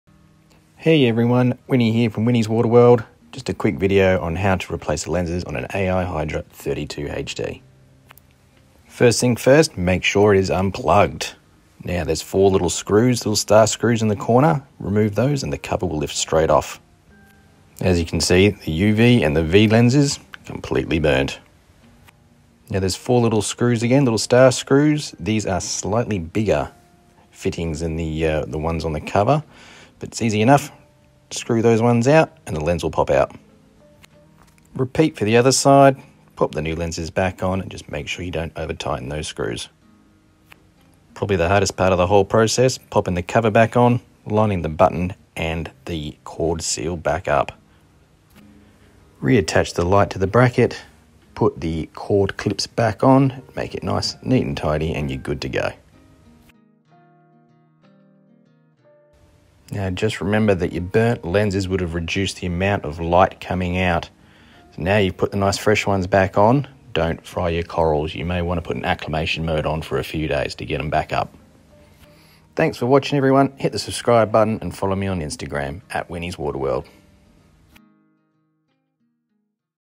Replacing AI Hydra 32HD lenses.
Quick and easy video to follow if you need to replace the lenses on your AI Hydra 32HD.